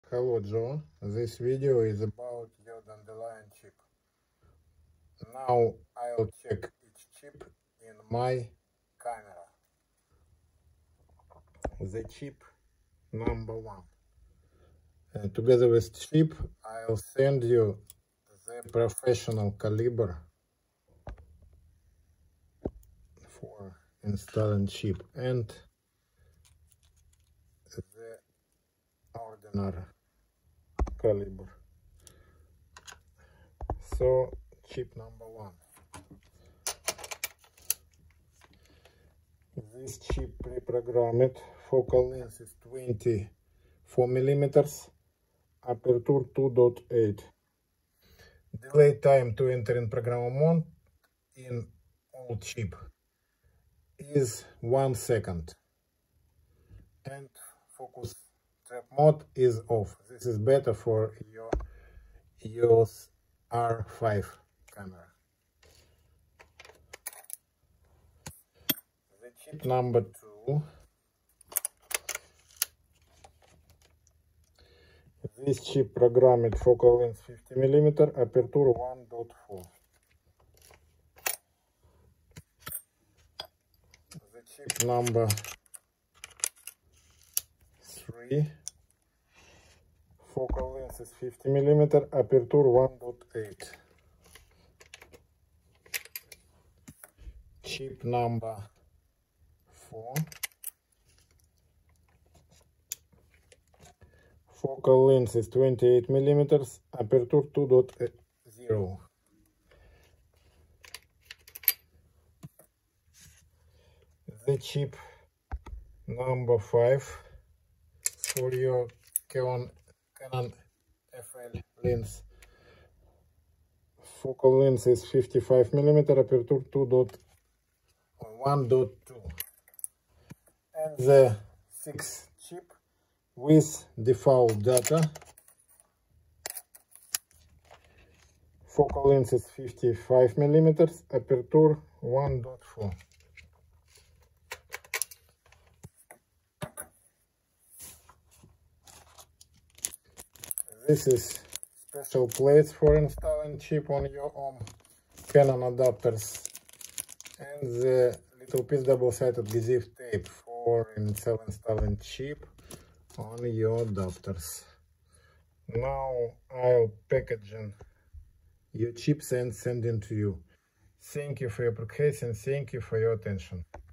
Canon Dandelion chip. 6 pcs. (For Joe H.)
Canon Dandelion chip.  6 pcs.
For:
1. Olympus OM 24mm f2.8
2. Olympus OM 50mm f1.4
3. Olympus OM 50mm f1.8
4. Olympus OM 28mm f2.0
5. Canon FL 55mm f1.2
6 Default 55mm f1.4
For direct purchase write to me:
Andrii Kurianov
Ukraine